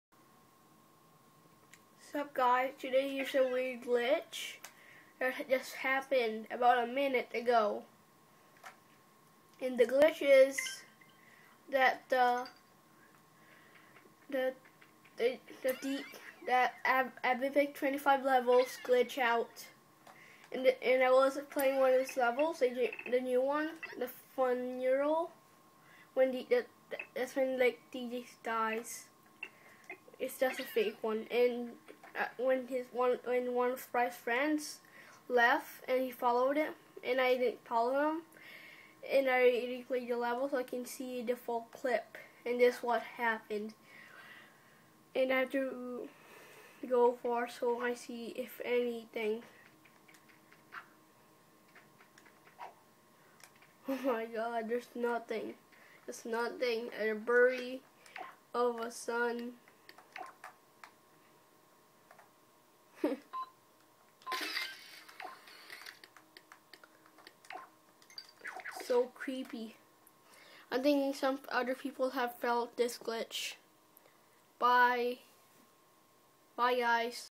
Weird Glitch
I was playing with my friend and one of his friend left I didnt folliw but bryce did I replayed the lvl for the rest of the lvl but it turned into a empty lvl!.